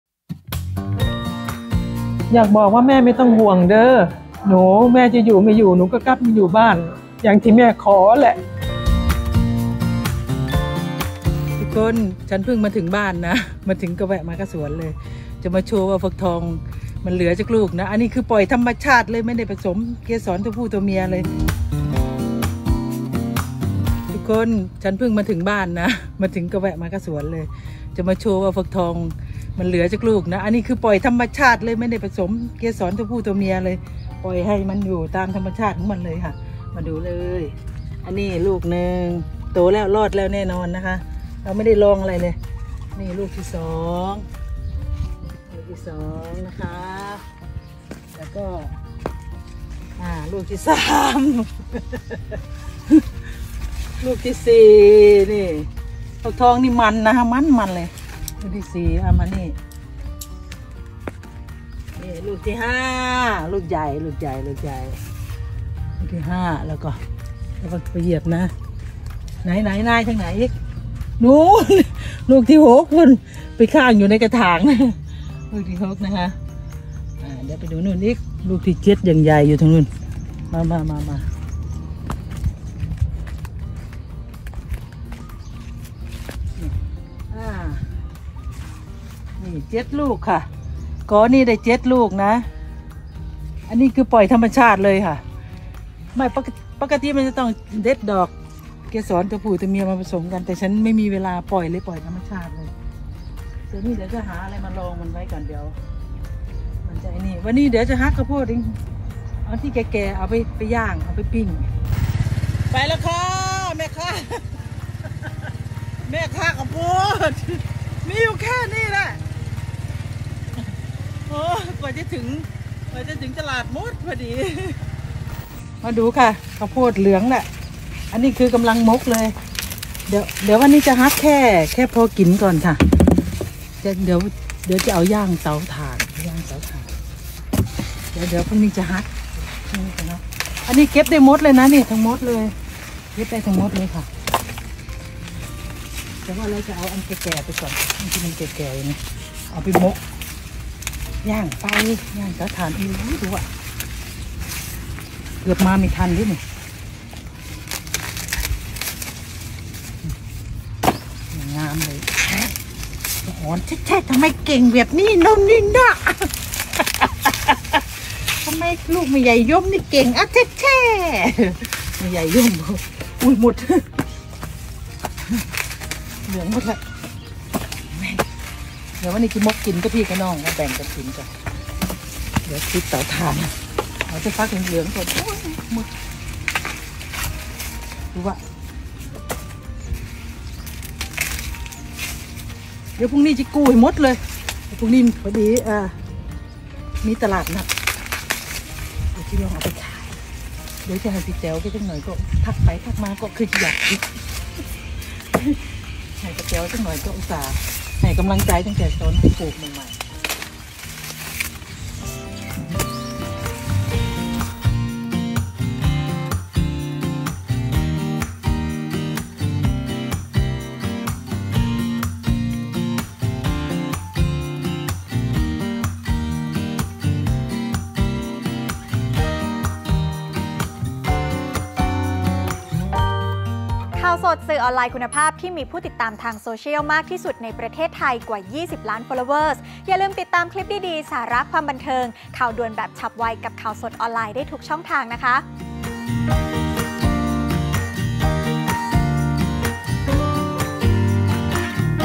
ทำตามที่แม่ขอ กลับโคราช สุนารี อยู่แบบเรียบง่าย : Khaosod - ข่าวสด
ทำตามที่แม่ขอไว้ ก่อนเสียชีวิต กลับโคราช สุนารี นักร้องดัง อยู่แบบเรียบง่าย ชีวิตสุขสม สบายใจ